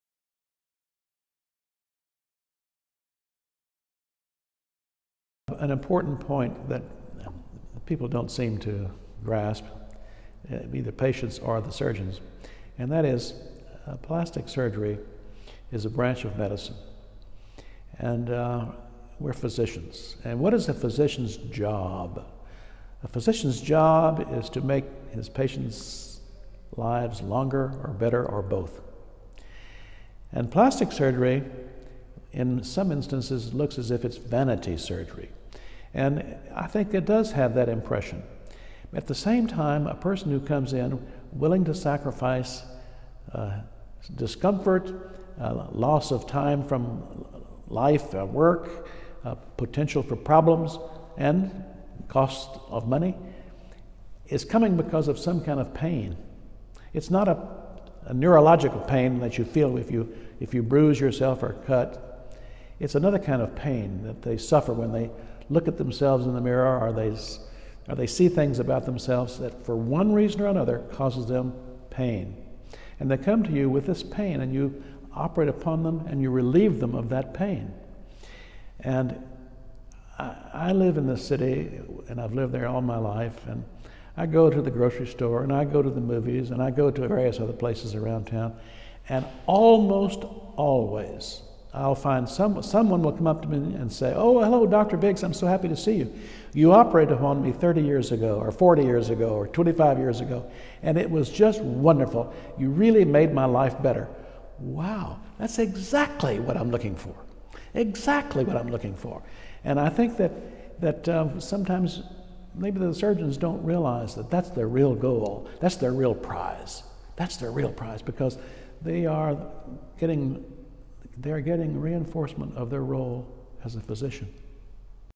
Dr. Thomas Biggs MD - About being a plastic surgeon
Dr. Thomas Biggs thinks that a physician's job is to make patients live longer, or better, or both. Plastic surgery is the same, patients want to leave behind the pain of not liking how they look. Dr. Thomas Biggs MD at the 2017 POLYTECH Excellence Master Class in Dubrovnik, Croatia.

Instagram English: @polytechhealth
Instagram Español: @polytechhealth_es
Instagram Deutsch: @polytech_de
X: @polytechhealth